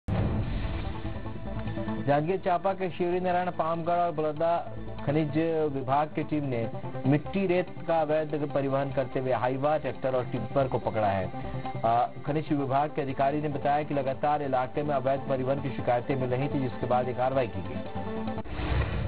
Janjgir : खनिज का अवैध परिवहन-उत्खनन, 15 वाहन जब्त, खनिज विभाग की टीम ने की कार्रवाई
Video from राजकुमार साहू, पत्रकार